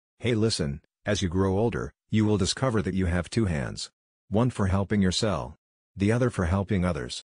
Life's Guiding Principle: Two Hands, One Purpose
“The best classroom in the world is at the feet of an elderly person.” -Andy Rooney

Older people can be a source of wise words and sage advice on dealing with and enjoying life to its fullest.

Wisdom from elderly people is often better than strength.

life lessons from an 80-year-old man. Many of the life lessons relate to our ability to process and handle emotions.

🌟 Embrace the Wisdom of Age 🌟

In this brief yet profound YouTube short, we explore the timeless wisdom of age. "As you grow older, you will discover that you have two hands, one for helping yourself, the other for helping others." Join us on a journey of introspection and inspiration as we reflect on the value of lending a hand, both to ourselves and to those around us.

🤝 Discover the power of balance, the beauty of self-care, and the joy of helping others in this enlightening moment. Life's lessons, condensed into a powerful quote, remind us of the true essence of living.

How has this principle impacted your life? Let's create a positive ripple effect of kindness and support together.